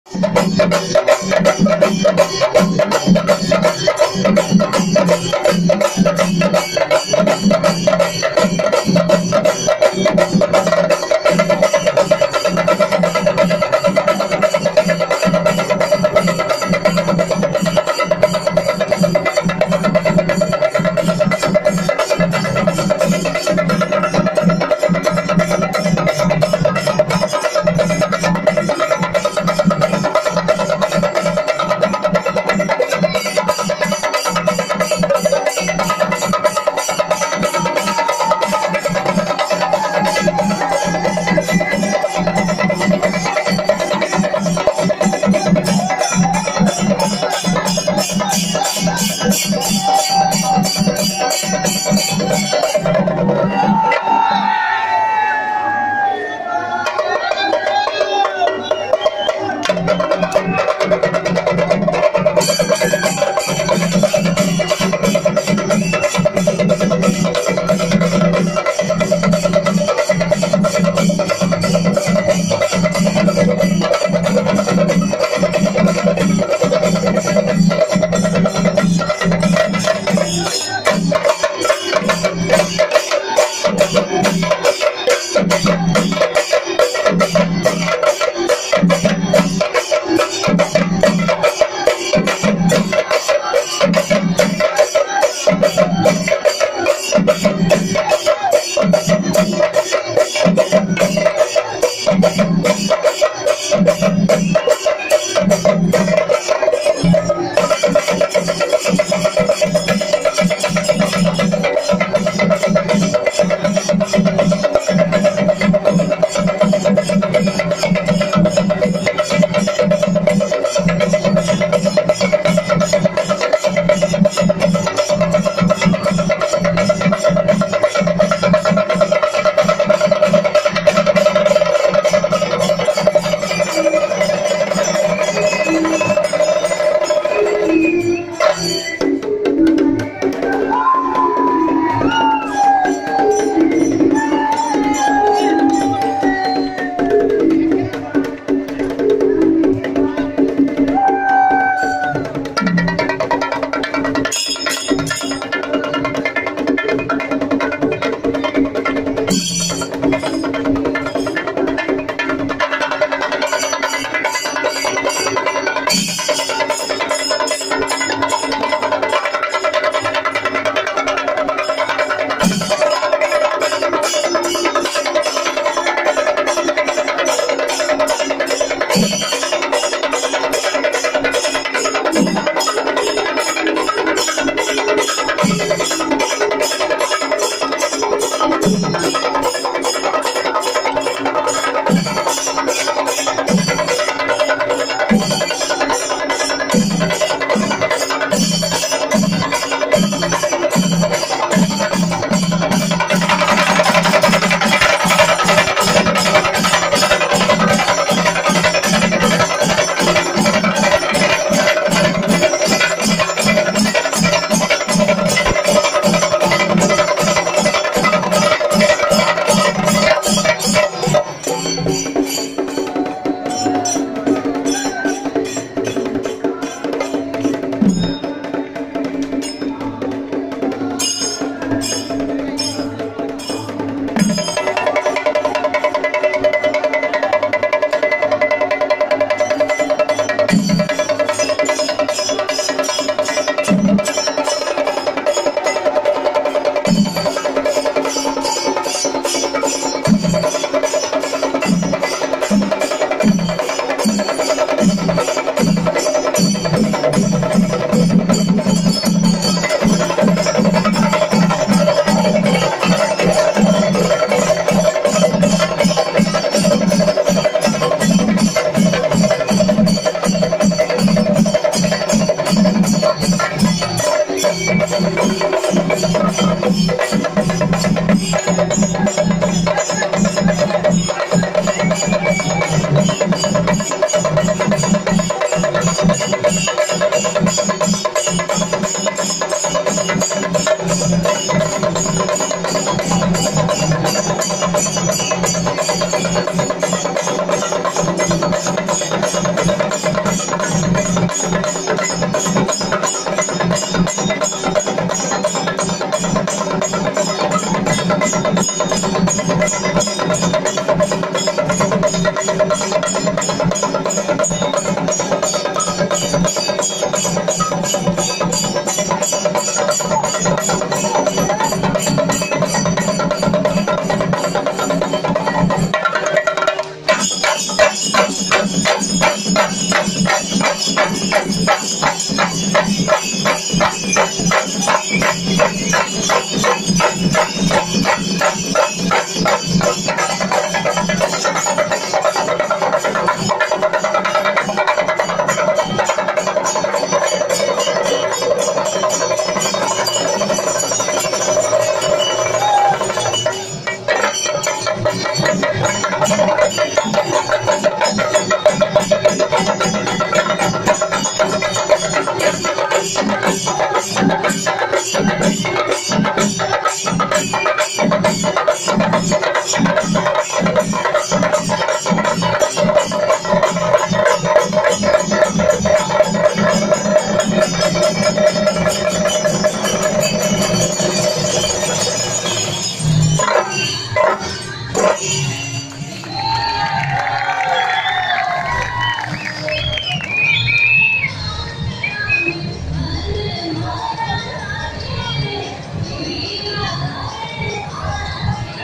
Kilukkam Vadhya Kala Sangam Chendamelam at Sree Hariharasuda Kshethram, Palarivattom Kochi
Awesome Performance.